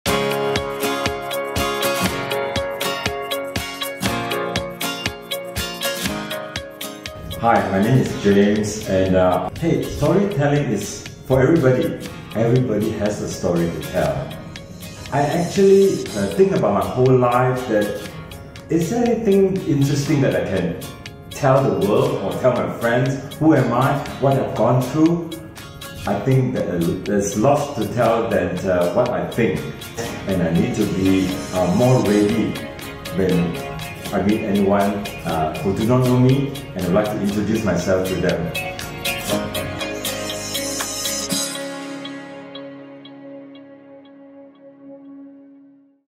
Participant Testimony – James
"Everybody has a story to tell. Is there anything interesting that I can tell the world or tell my friends what I've gone through?

There are lots to tell than what I think. I need to be more ready when I meet anyone who does not know me and to introduce myself to them."
James
Life Coach-in-Training

James participated in a Narrative Storytelling workshop facilitated for Morphos International Life Coaches.

Join us to learn how you can communicate better with your stakeholders and touch lives through digital storytelling.